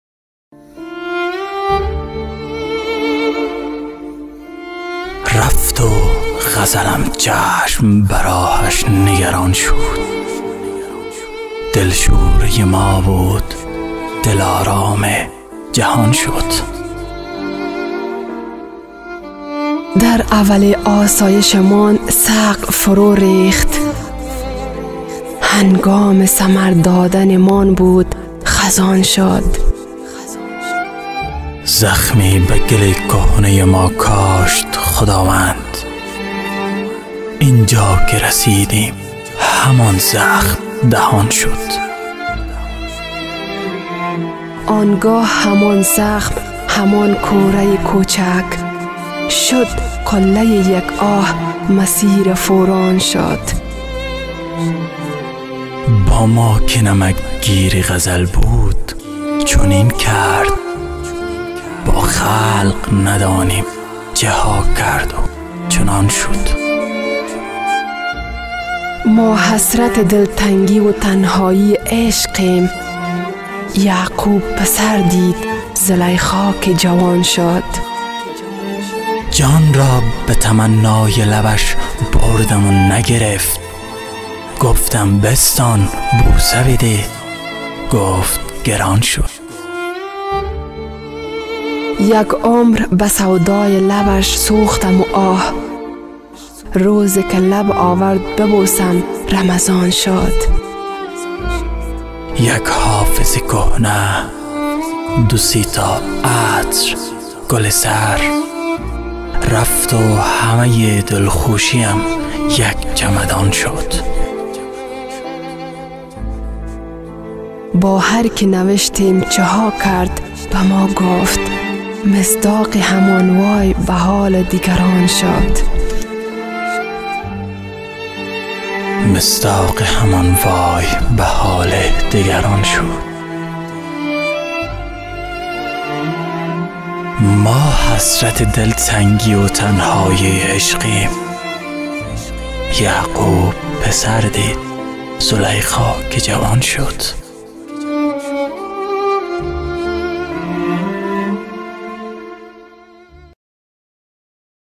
رفت و غزلم چشم به راهش نگران شد | دکلمه شعر عاشقانه
This channel will be for your Entertainment bellow will be the content

رفت و غزلم چشم به راهش نگران شد

دلشوره ی ما بود، دل آرام جهان شد

در اوّل آسایش مان سقف فرو ریخت

هنگام ثمر دادن مان بود خزان شد

زخمی به گل کهنه ی ما کاشت خداوند

اینجا که رسیدیم همان زخم دهان شد

آنگاه همان زخم، همان کوره ی کوچک،

شد قلّه ی یک آه، مسیر فوران شد

با ما که نمک گیر غزل بود چنین کرد

با خلق ندانیم چه ها کرد و چنان شد

ما حسرت دلتنگی و تنهایی عشقیم

یعقوب پسر دید، زلیخا که جوان شد

جان را به تمنّای لبش بردم و نگرفت

گفتم بستان بوسه بده، گفت گران شد

یک عمر به سودای لبش سوختم و آه

روزی که لب آورد ببوسم رمضان شد

یک حافظ کهنه، دو سه تا عطر، گل سر

رفت و همه ی دلخوشی ام یک چمدان شد

با هر که نوشتیم چه ها کرد به ما گفت

مصداق همان وای به حال دگران شد